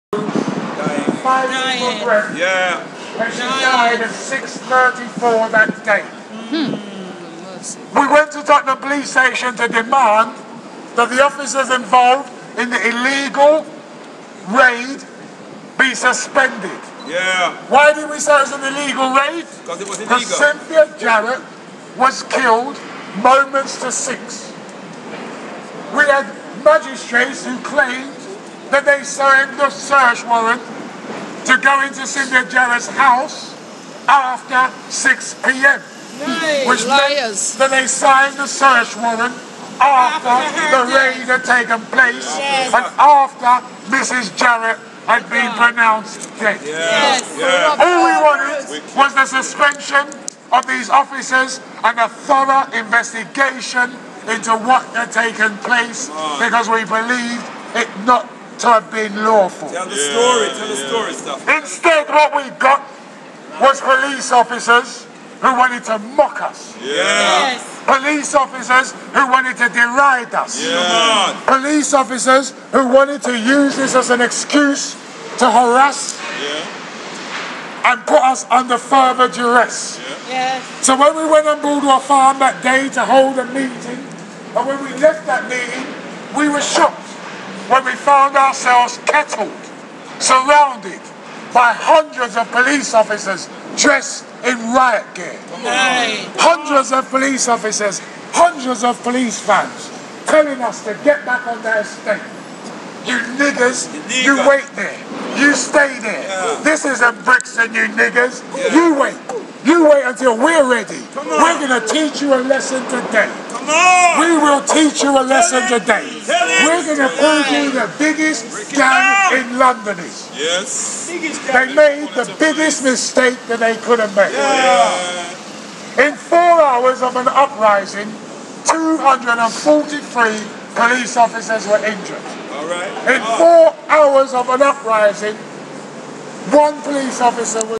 PC Blakelock Murder Trial Photographic Evidence does not show Nicky Jacobs Stabbing
Photographic evidence has come under close scrutiny in the Trial of  PC Blakelock Murder as further doubt is cast on data as reliable evidence; leaving the Police again in a very embarrassing position.  Officers were again left in an uncomfortable position stuttering and head hung in the dock unable to answer basic questions and admitting to major errors in basic scheduling of photos.  Other revelations that all the photographic evidence was taken by only Police Officers and at some point there was a break claimed as damage and the timing and labelling  of such evidence began to shock the court room as it appeared that they had only photographed one section of the entire Broadwater Farm and lost all opportunity to gather information leaving the case now open to anyone be responsible and not being identified the Judge had to intervene and ask the witness to discuss in private with both sides the intimacy and settle the confusion and missing evidence and timings and photos.

Case resumes today to try to untangle the erroneous use of photographs that left the public gallery exhausted and leading no where nearer to resolving the seriousness of this case and again was pointing to Nicky Jacobs innocence rather than a conviction as the Defence declared the timing for the window of opportunity for Nicky Jacobs  going by even the photographic current evidence does not fit the crime as well as doubts to his exact identity and if the same reasoning the Prosecution was applied to the photographic evidence would mean countless other suspects would be accountable.

Video101